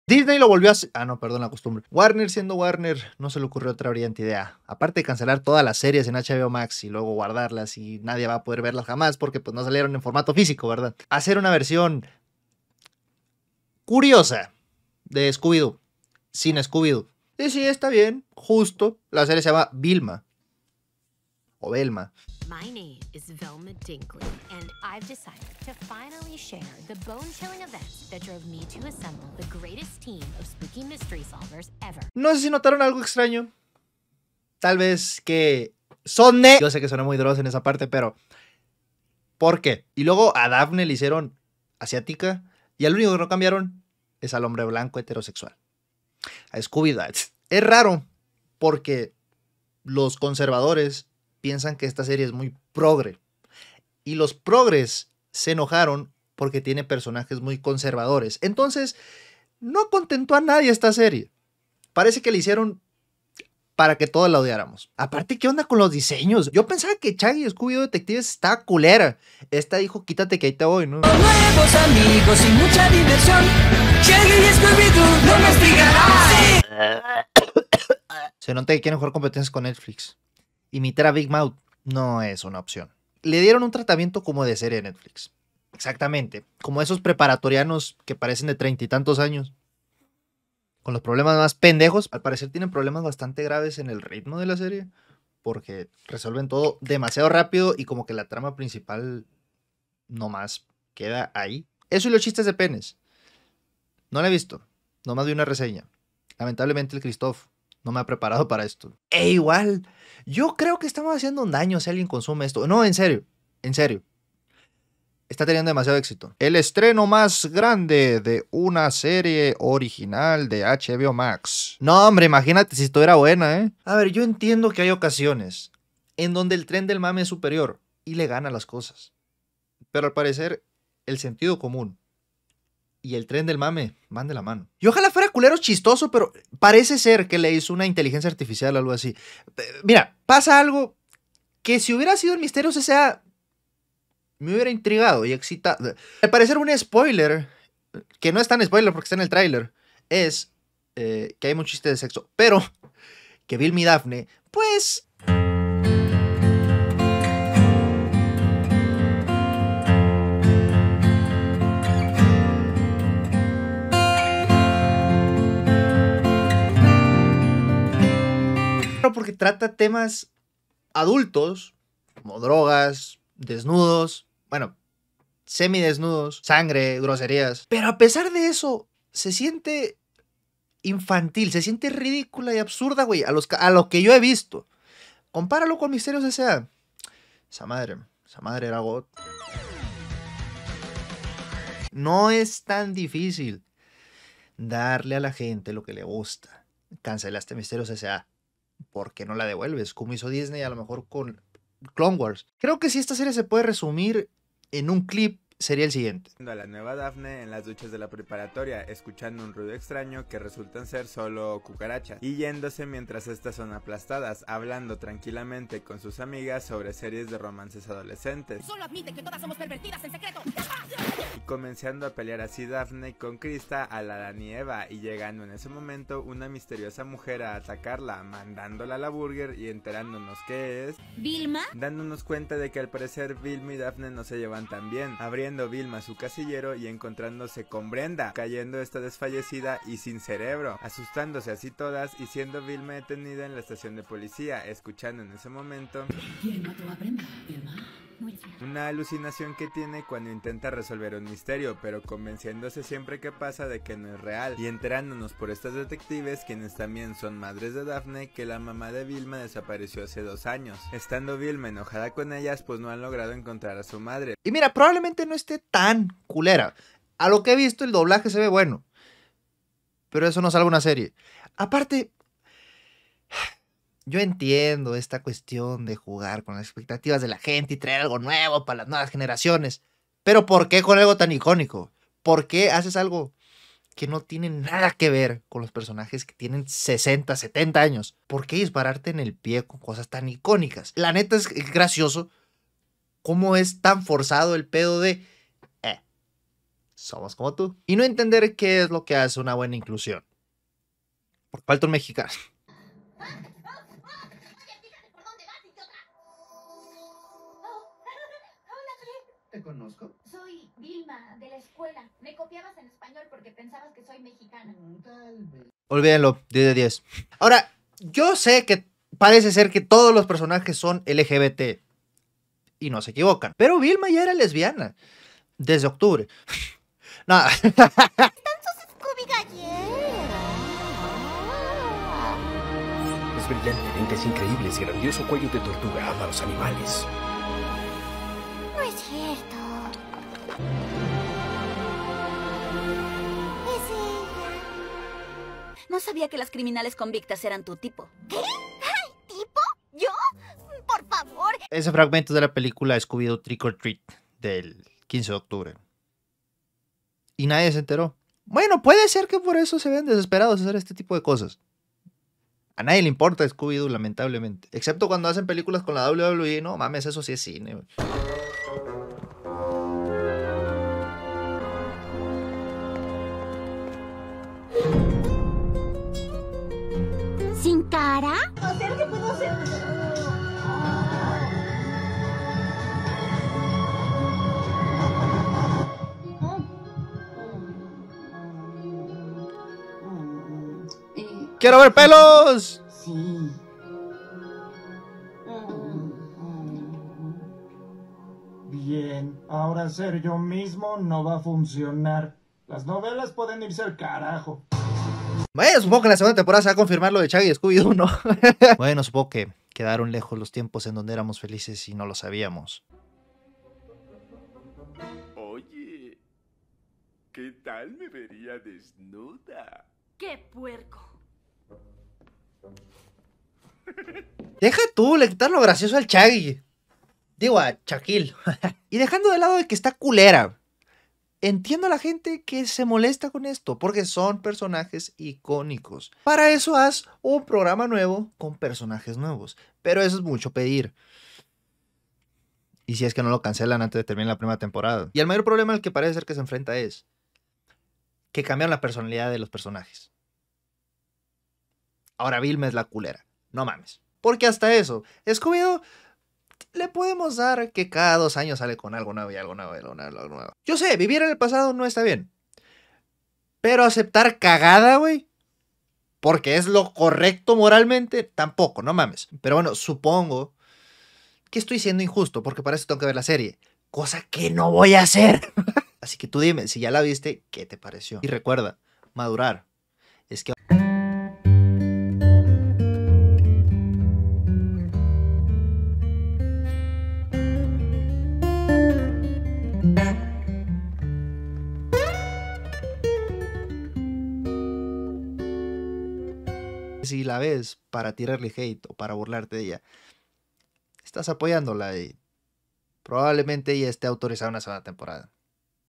Velma es horrible.
La serie de Velma de hbo es terrible. scooby doo papa